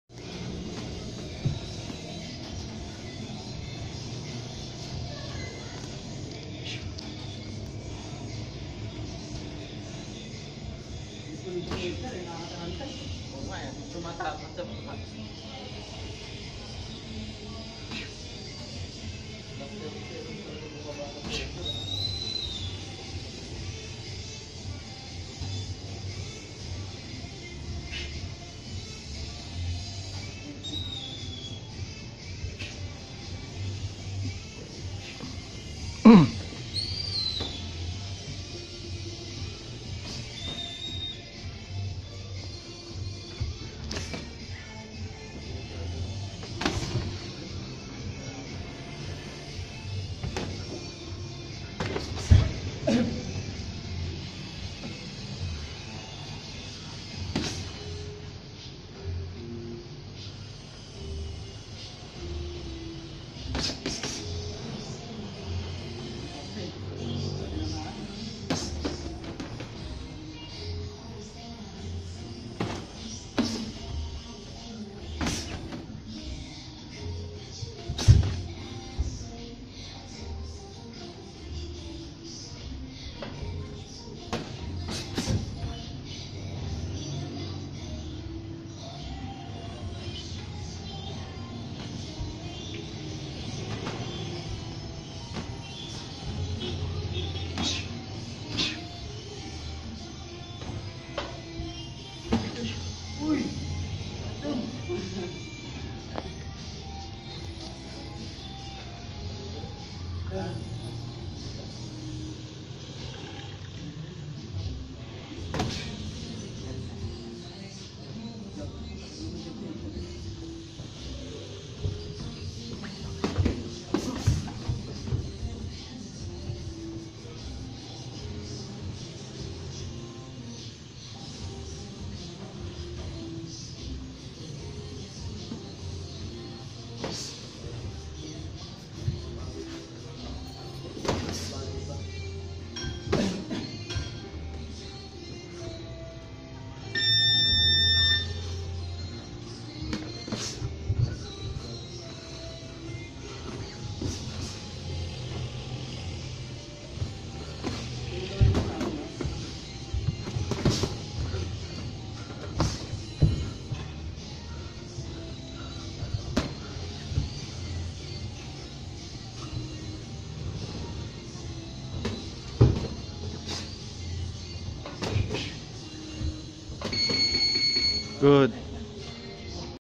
syrus decipolo on white trunks sparring with the living legend coach edgar matobato
training for upcoming fight